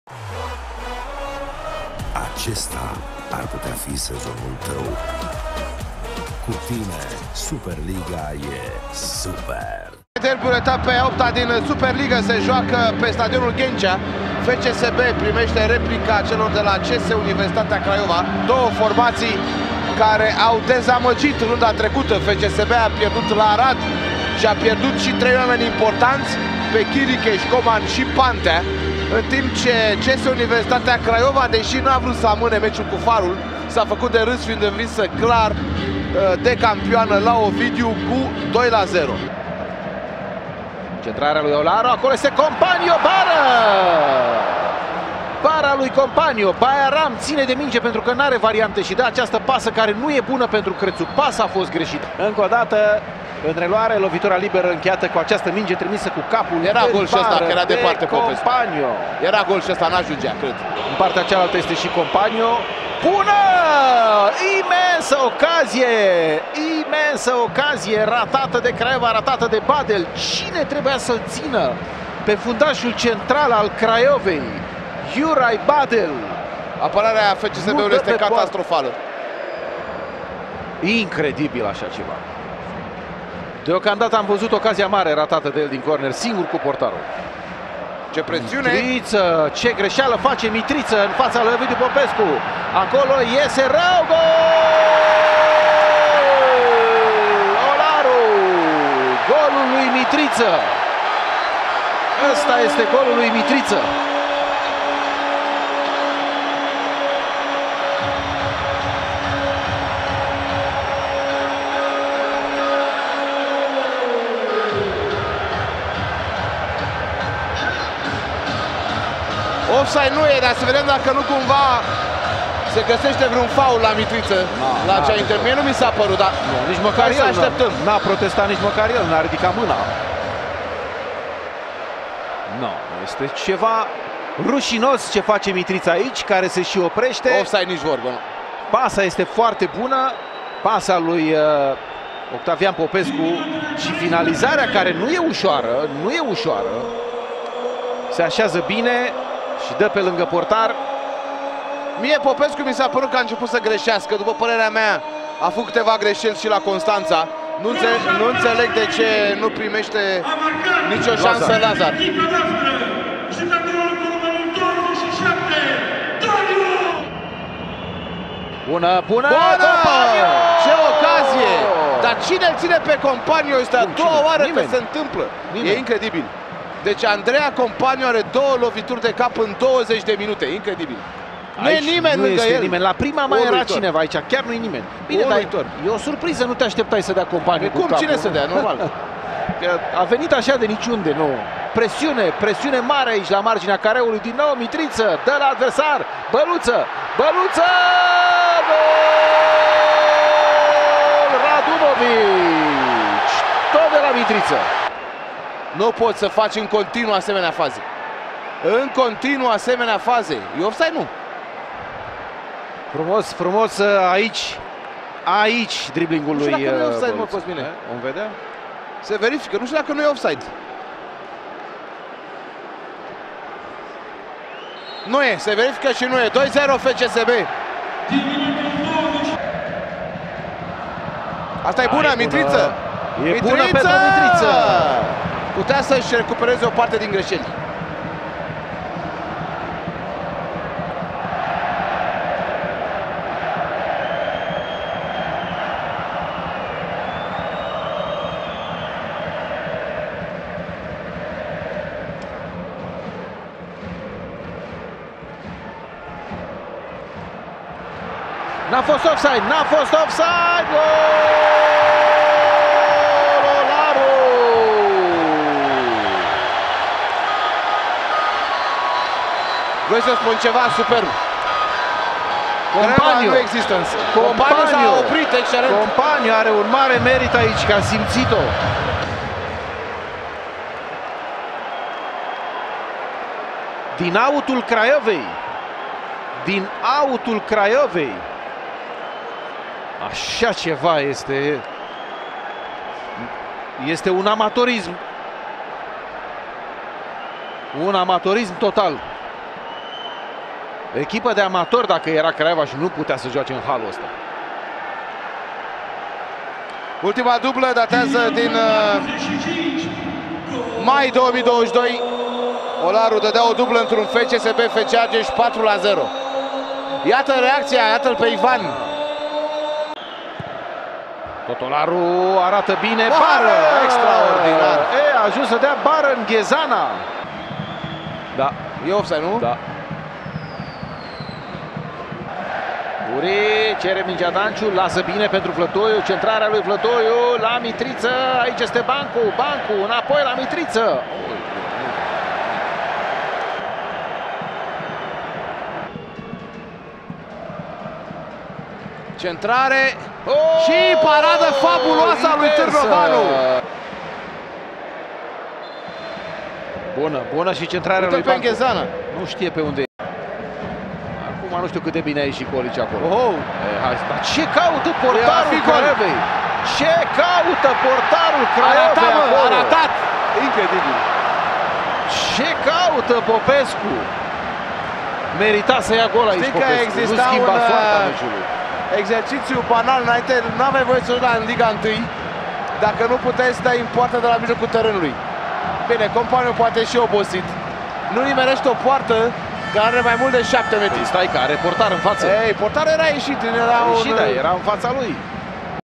REZUMAT | FCSB - Univ. Craiova 3-0. Victorie mare FCSB, greşeli uriaşe ale oltenilor
FCSB - Universitatea Craiova 3-0

Au marcat: Olaru 13, 45, Radunovic 21

Echipele de start:

FCSB: Târnovanu-  Creţu, Dawa, Ngezana, Radunovic-  Ov. Popescu, Şut, Olaru- Băluţă, Compagno, Oct. Popescu

Rezerve: Udrea, Panţîru, Haruţ, Miculescu, Gogescu, Lixandru, Toma, Radaslavescu, Djokovic

Universitatea Craiova: Popescu- Ndong, Zajkov, Badelj, Bancu- Kurtic, Screciu, Creţu, Baiaram, Mitriţă- Ivan

Rezerve: Lazar, Raul Silva, Mateiu, Căpăţînă, Houri, Vlădoiu, Danciu, Işfan, Koljic